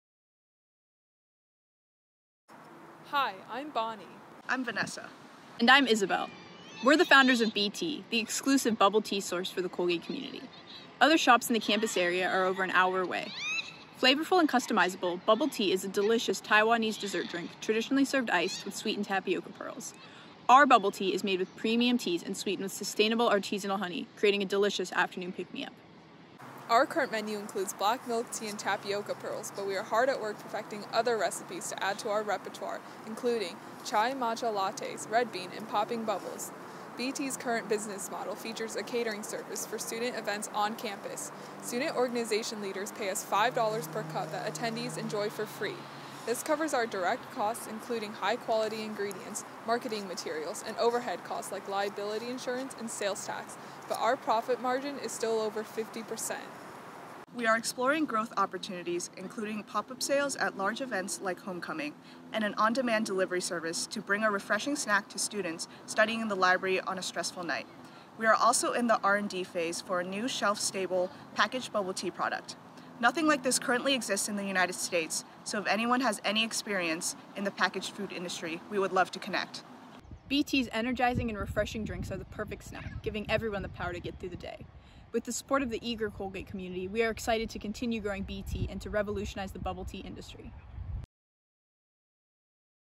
bee tea | demo day 2020
bee tea is a sustainable, pop-up bubble tea shop on the Colgate campus. We source premium ingredients to create delicious, honey-sweetened drinks for everyone to share.